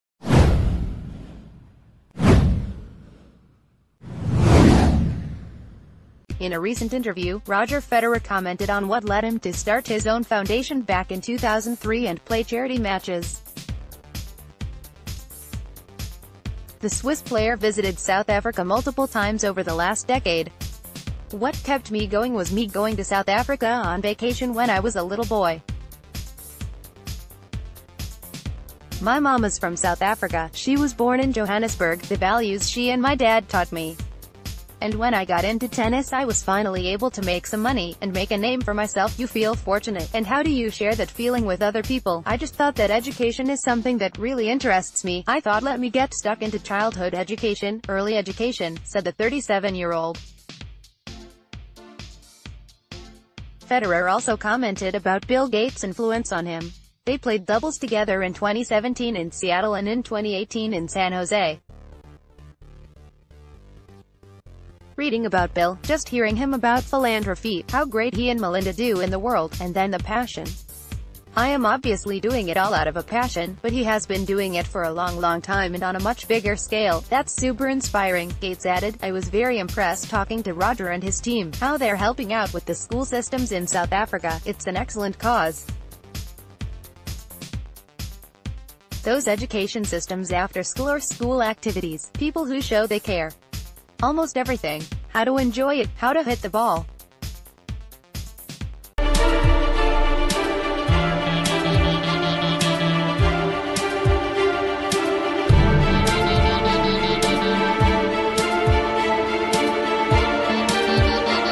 Roger Federer explains why he helps kids in Africa through his Foundation
In a recent interview, Roger Federer commented on what led him to start his own Foundation back in 2003 and play charity matches. The Swiss player visited South Africa multiple times over the last decade. "What kept me going was me going to South Africa on vacation when I was a little boy. My mom is from South Africa, she was born in Johannesburg, the values she and my dad taught me. And when I got into tennis I was finally able to make some money, and make a name for myself, you feel fortunate,...